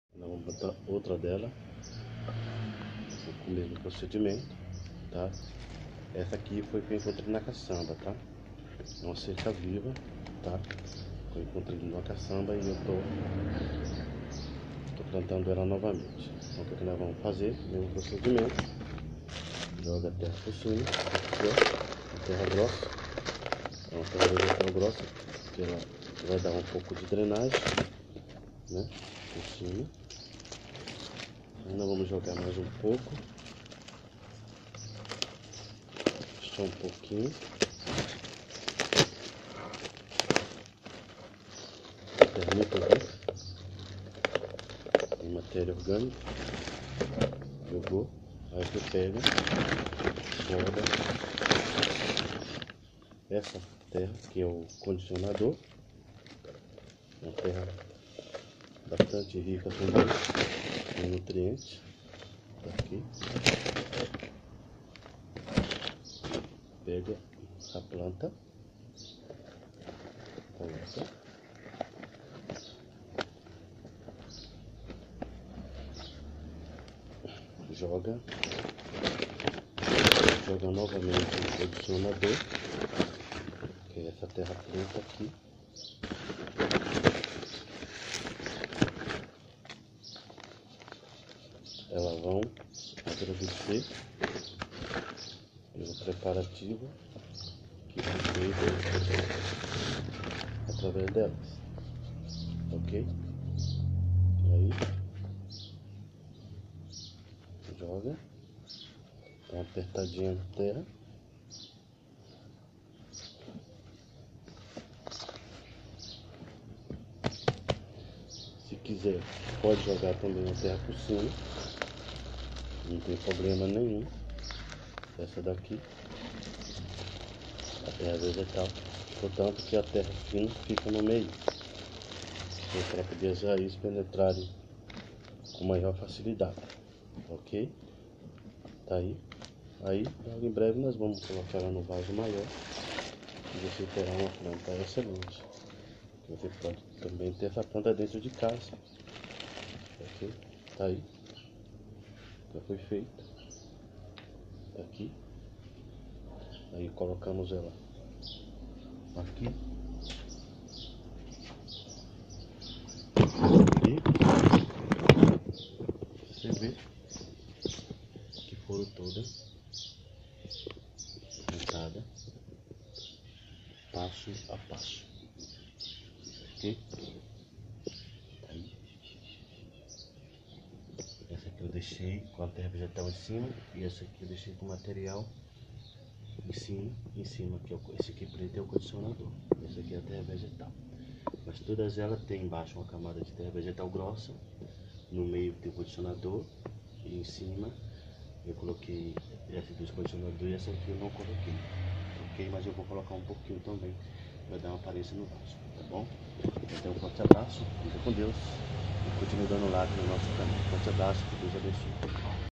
A CLUSIA SENDO PLANTADA.
Essa planta foi encontrada em uma casamba de lixo, o sol estava muito quente, porém peguei elas no dia 12/08/2017, e foi replantadas e recuperadas no dia 14/08/2017,e elas estão muito felizes pelas suas folhas ver uma feliz. Ela se multiplica por estaquia.